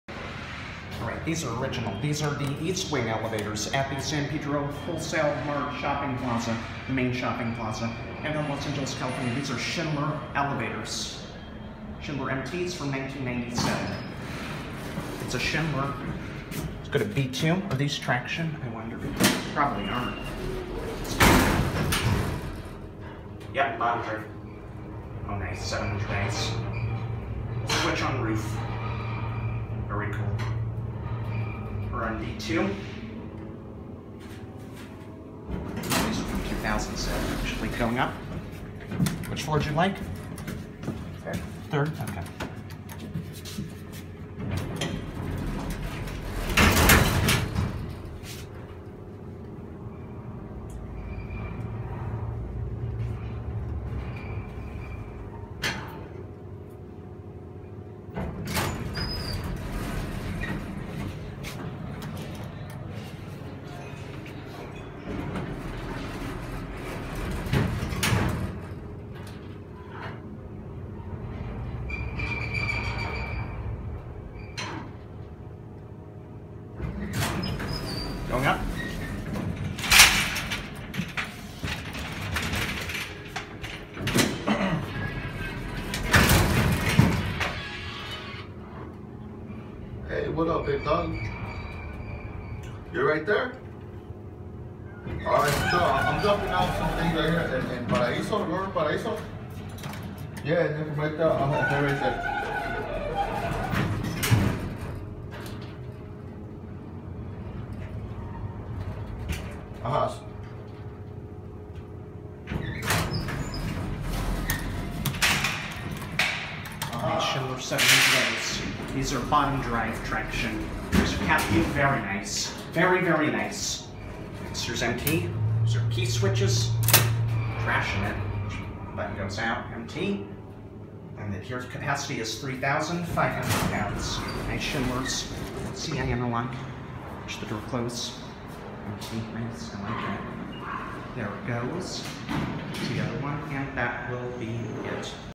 Schindler MT 700A Traction Elevators @ San Pedro Wholesale Mart, Los Angeles, CA
VERY AWESOME set of elevators here!!!:D Installed in 1997.
Speed: 400FPM Geared Overhead Traction.